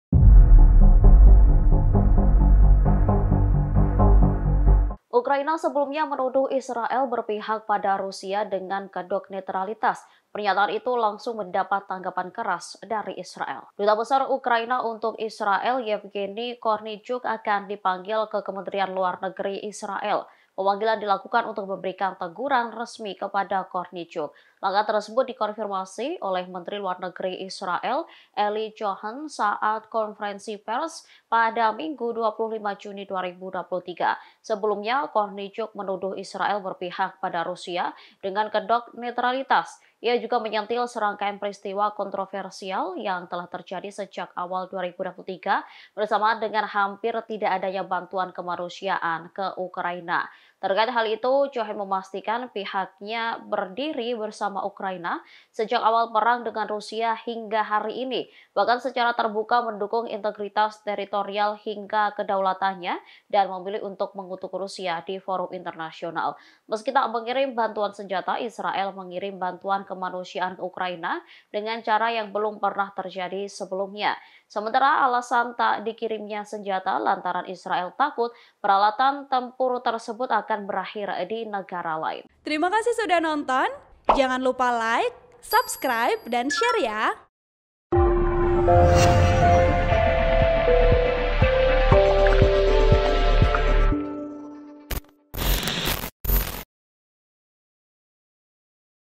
Ukraina DISEMPROT Israel, Dinilai Bicara ASAL-ASALAN Hubungan dengan Rusia, Pejabat Kiev Dipanggil
TRIBUN-VIDEO.COM - Ukraina sebelumnya menuduh Israel berpihak pada Rusia dengan kedok netralitas.

Pernyataan itu langsung mendapat tanggapan keras Israel.

Duta Besar Ukraina untuk Israel, Yevgeni Kornichuk akan dipanggil ke Kementerian Luar Negeri Israel.

Pemanggilan dilakukan untuk memberikan teguran resmi kepada Kornichuk.

Langkah tersebut dikonfirmasi oleh Menteri Luar Negeri Israel Eli Cohen, saat konferensi pers hari Minggu (25/6/2023).

Sebelumnya, Kornichuk menuduh Israel berpihak pada Rusia dengan kedok netralitas.

Ia juga menyentil serangkaian peristiwa kontroversial yang telah terjadi sejak awal 2023, bersamaan dengan hampir tidak adanya bantuan kemanusiaan ke Ukraina.

Terkait hal  itu, Cohen memastikan, pihakmua  berdiri bersama Ukraina sejak awal perang dengan Rusia hingga hari ini.

Bahkan, secara terbuka mendukung integritas teritorial hingga kedaulatannya dan memilih untuk mengutuk Rusia di forum internasional.

Meski tak mengirim bantuan senjata, Israel mengirim bantuan kemanusiaan ke Ukraina dengan cara yang belum pernah terjadi sebelumnya.

Sementara alasan tak dikirimnya senjata lantaran Israel takut peralatan tersebut akan berakhir di negara lain.

(Tribun-Video.com/rt.com)

Artikel ini telah tayang di rt.com dengan judul
Israel to reprimand Ukrainian diplomat